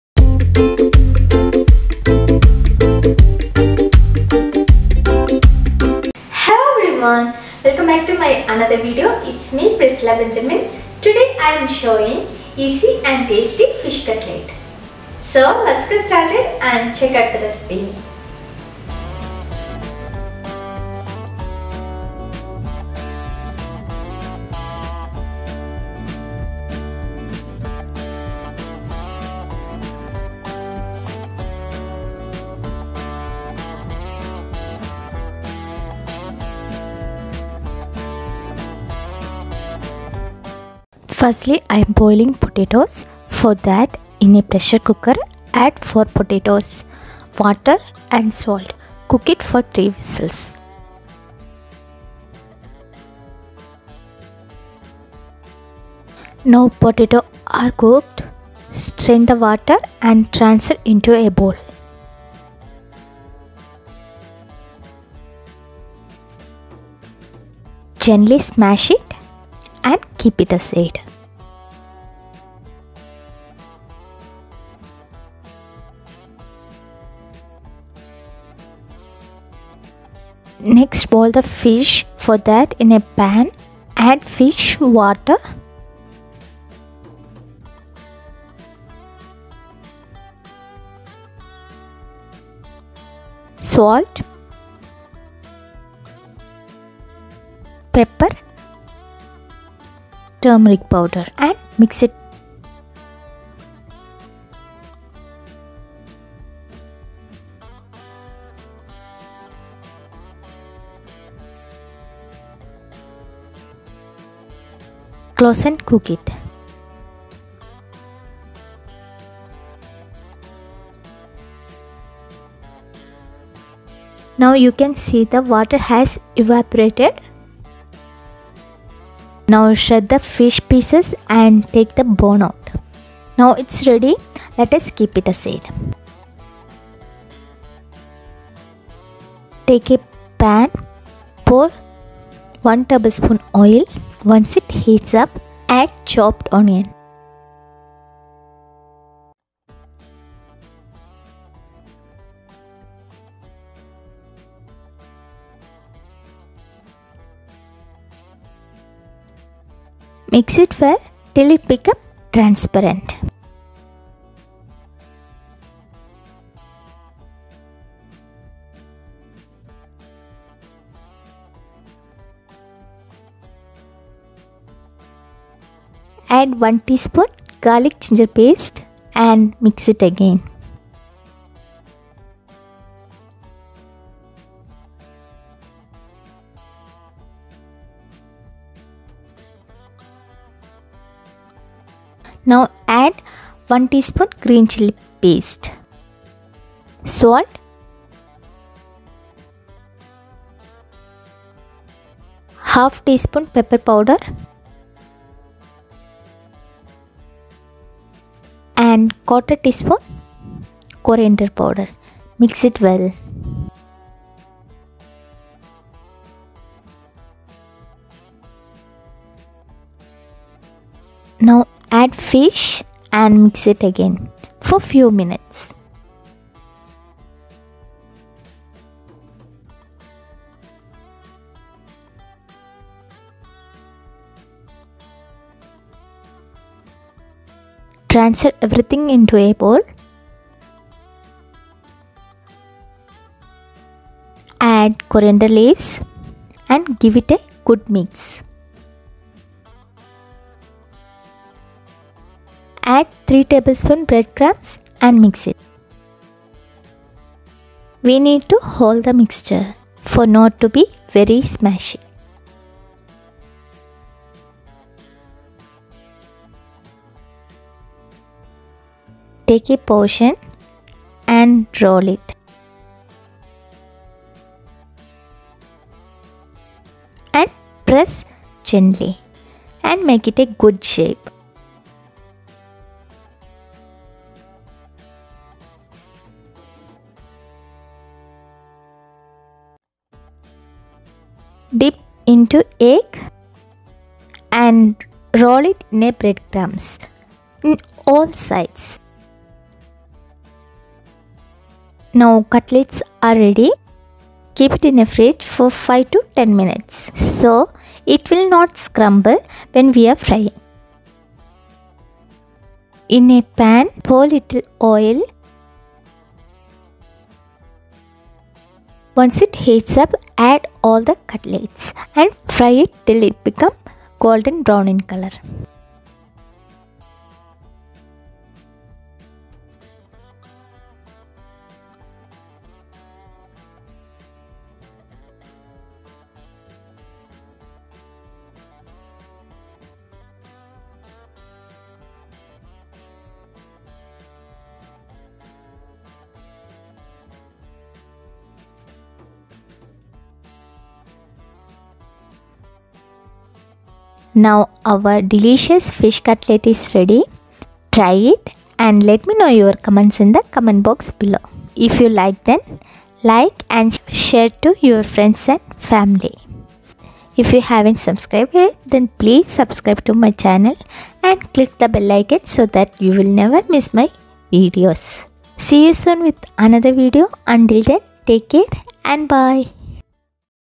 Fish Cutlet recipe | How to make fish cutlet |Fish Recipes
Episode-94

fish cutlet recipe
Ingredients
Fish-250gm
Onio-1
Potato-4
Egg-2
Garlic &ginger paste-1tsp
Breadcrumbs
greenchilli paste-1tsp
pepper powder-1/2tsp
salt to taste
oil
turmeric powder

Fish cutlets recipe